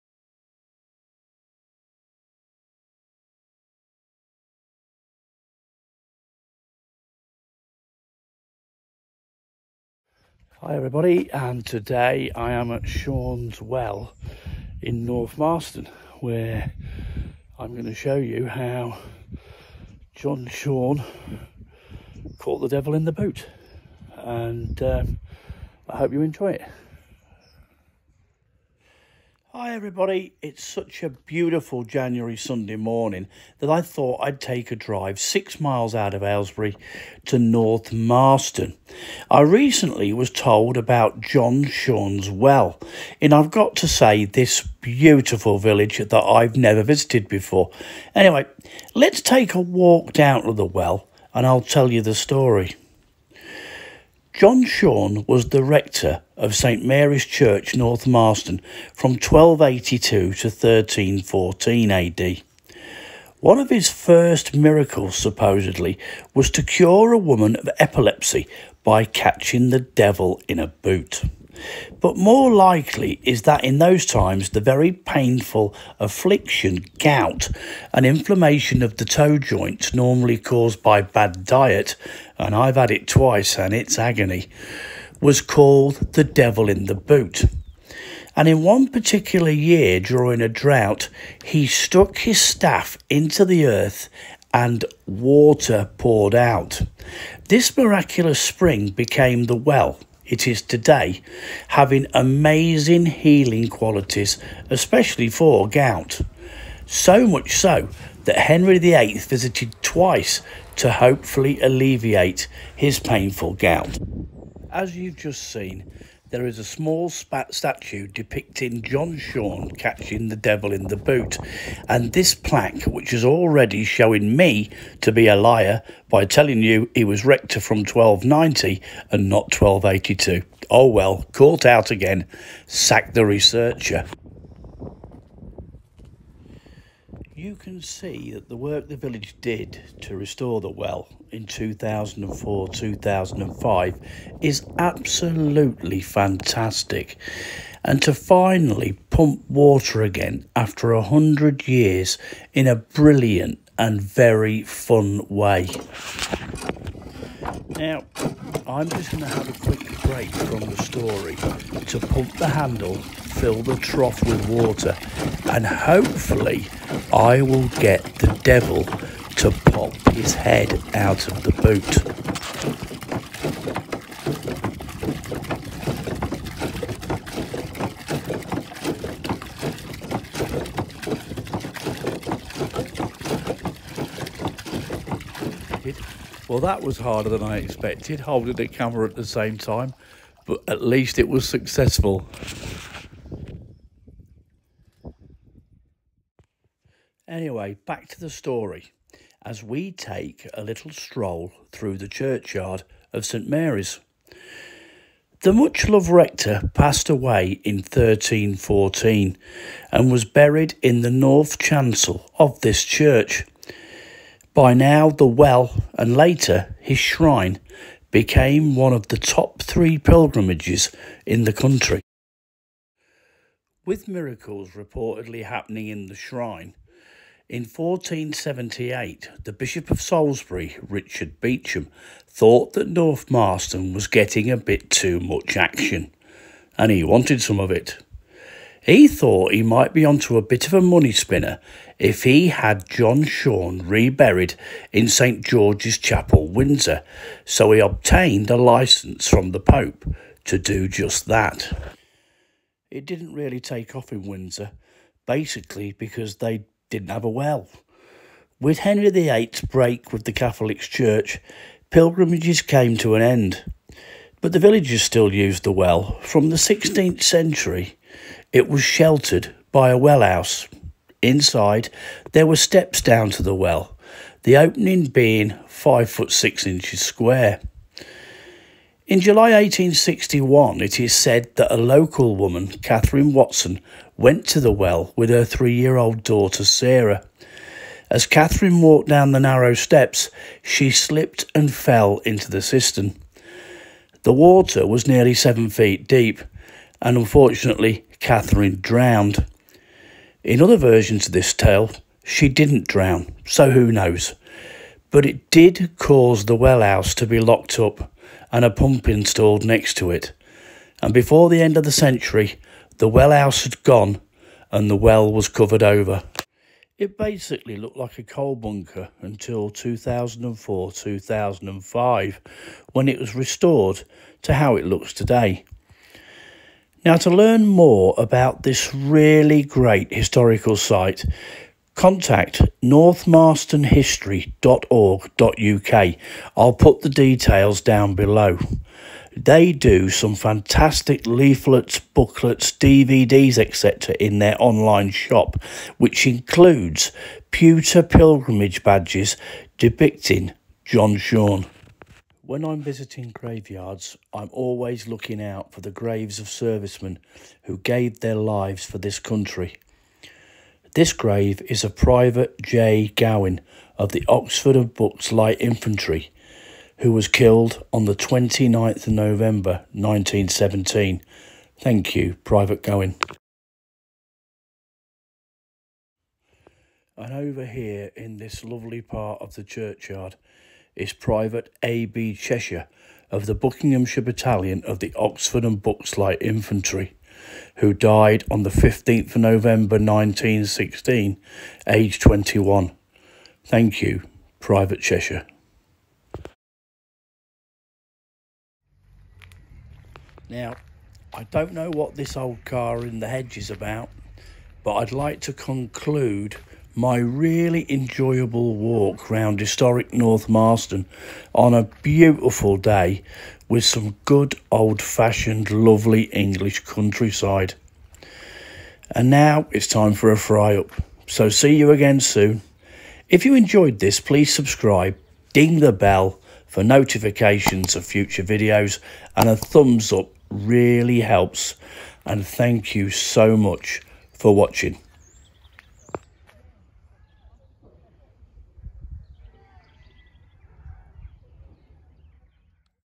The Devil in the boot , John Schorne’s well North Marston Buckinghamshire
A little Sunday trip out to see the famous pilgrimage site of John Schornes well !thanks too